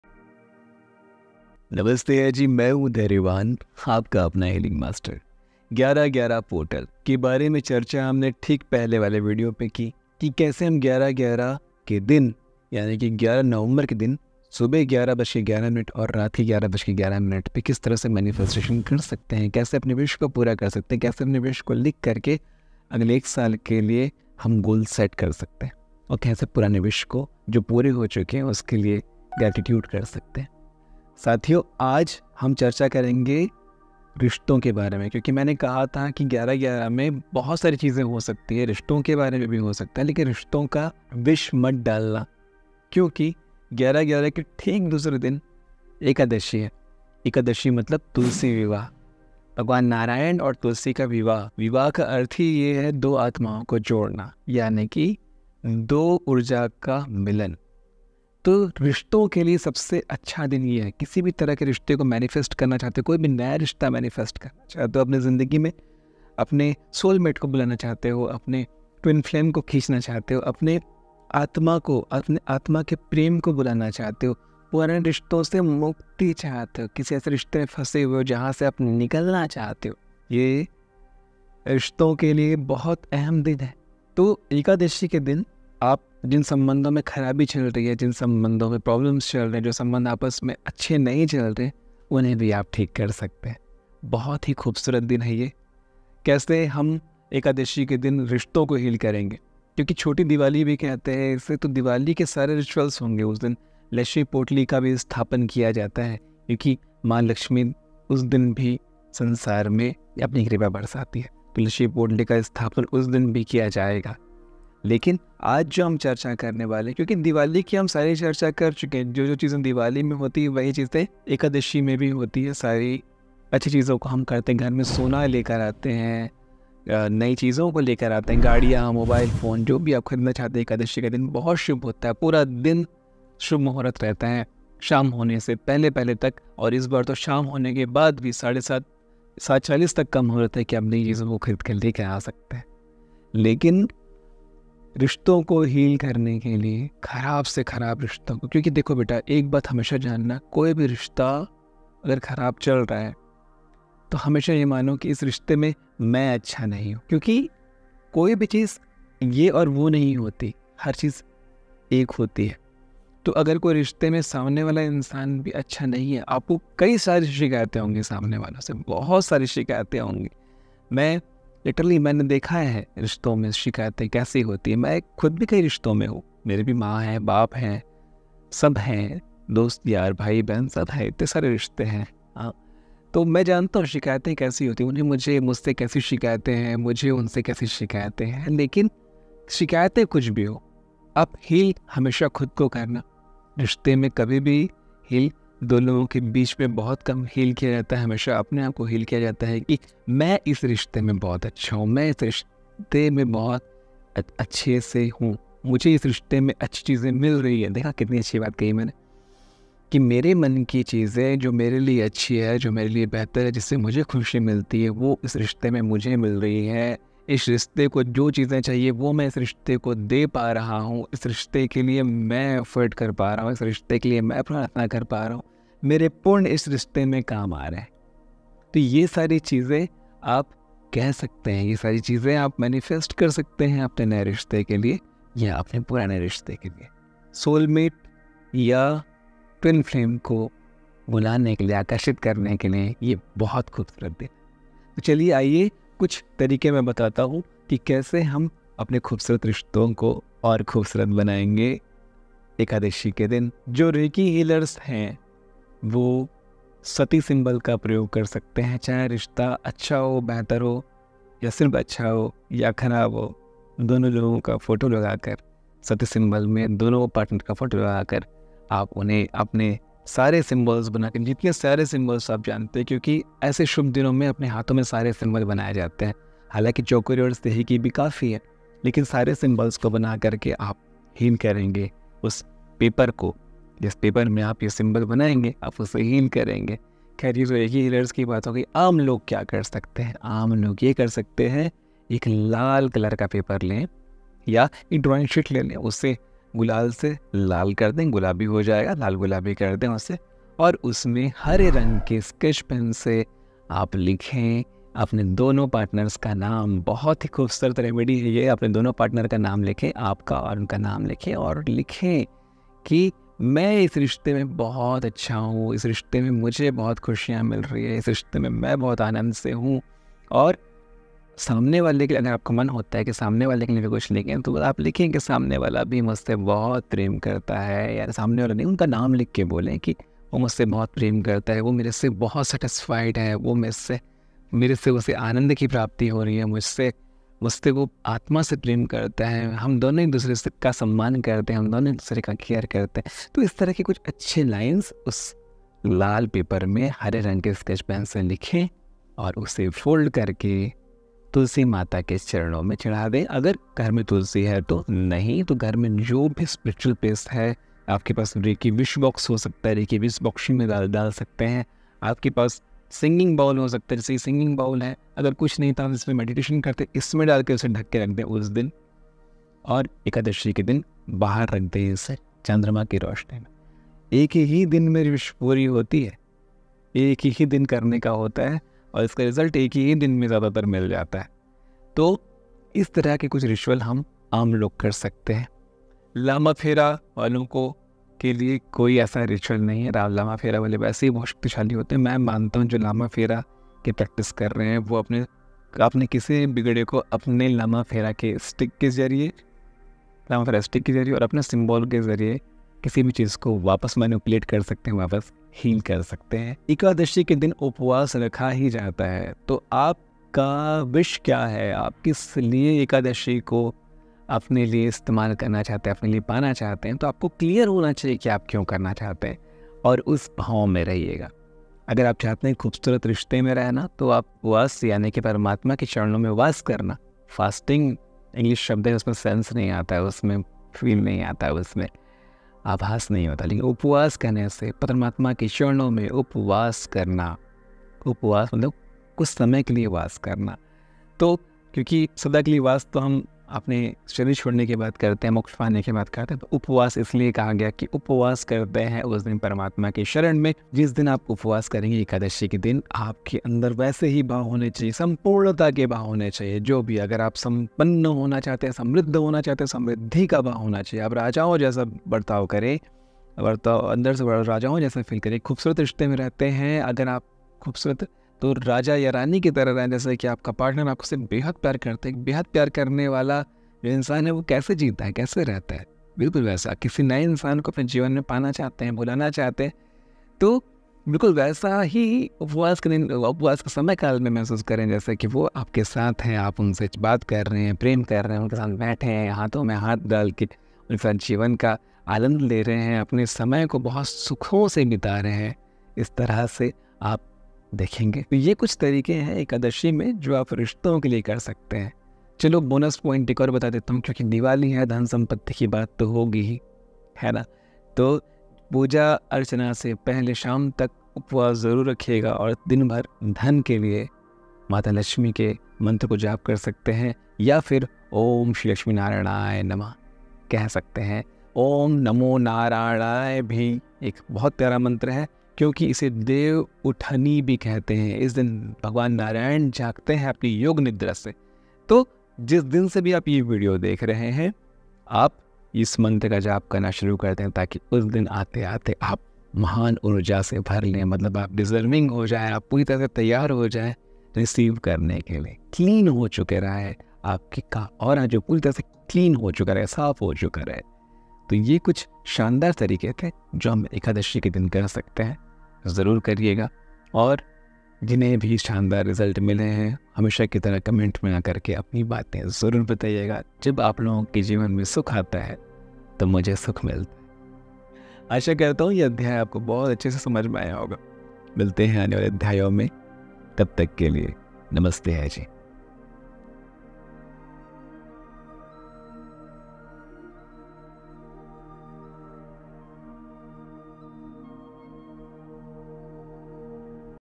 Ekadashi Rituals for Strong Relationships, जुड़ेंगे नयें सुंदर रिशतें॥ By Dhairyawan
Video Description

In this enlightening video, Dhairyawan Sir shares vital insights into the significance of Ekadashi, particularly focusing on the Tulsi Vivah ritual. He reveals how to harness these spiritual practices to manifest love, harmony, and connection in your relationships. By understanding these sacred rituals, viewers will learn practical steps to enhance their bond with their partners and cultivate a fulfilling romantic life. Join us on this journey of love and spirituality!

Key Takeaways

Understanding Ekadashi: Discover what Ekadashi is and why it holds significance in the realm of love.

Tulsi Vivah Ceremony: Learn the steps involved in this sacred ritual aimed at strengthening relationships.

Manifestation Techniques: Explore how to effectively manifest positive energy in your relationship through traditional practices.

Step-by-Step Process

Prepare for Ekadashi:

Clean your living space and create a sacred atmosphere.

Gather materials for the ritual, including a Tulsi plant and offerings like fruits and sweets.

Tulsi Vivah Ceremony:

Set up the altar with the Tulsi plant, decorated with flowers and incense.

Perform a prayer or mantra dedicated to Lord Vishnu, inviting divine blessings on your relationship.

Offer the gathered items to the Tulsi plant, symbolizing the union of divine energies.

Visualize & Manifest:

Close your eyes and visualize your ideal relationship.

Speak affirmations or mantras that resonate with love and joy, sending out positive vibes.

Celebrate with Gratitude:

Conclude the ritual by expressing gratitude for your partner and the love you share.

Share prasad (sacred food) with your partner as a token of your love and devotion.

Resources Mentioned

Tulsi Plant: Known for its spiritual significance in Hindu culture, the Tulsi plant is said to embody purity and devotion. It helps in creating a positive atmosphere during rituals.

Incense Sticks: Used to purify the surrounding environment, incense sticks enhance spiritual energy and create a calming ambiance essential for rituals.

Mantras/Prayers: Specific mantras dedicated to Lord Vishnu can channel divine energy, focusing it on nurturing love and connectivity between partners.

SEO-Optimized Hashtags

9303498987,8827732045
Instagram मे जुड़िए